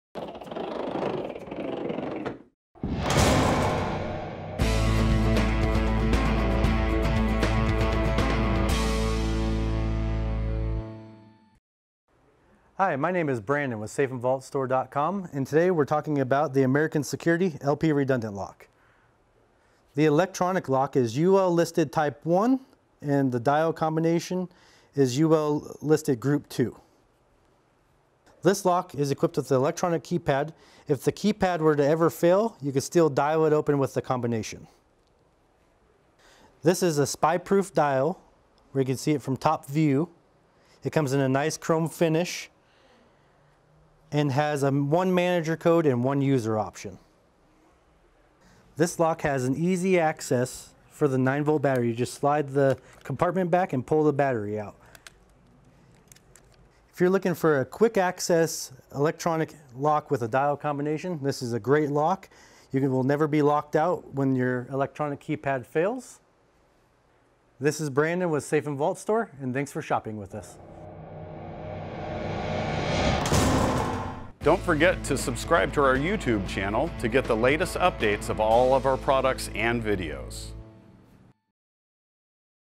LP Rotobolt EMP Proof Redundant Lock (Dial Combo & Digital Lock) Video
To learn more and purchase the LP Rotobolt EMP Proof Redundant Lock Retrofit Kit, go here:

Hi, my name is Brandon with Safe & Vault Store.com, and today we are talking about the American Security LP Redundant Lock.

The Electronic Lock is UL Listed Type 1 and the Dial Combination is UL listed Group II. This lock is equipped with an electronic keypad. If the keypad were to ever fail, it would still dial open with the combination.

This is a spyproof dial where you can see it from the top view. It comes in a nice chrome finish and has 1 manager code and 1 user option.

This lock has easy access for the 9V battery. You just slide the compartment back and pull the battery out.

If you're looking for a quick access electronic lock with a dial combination lock override, this is a great lock. You will never be locked out if your electronic keypad fails.

This is Brandon with Safe & Vault Store. Thanks for shopping with us.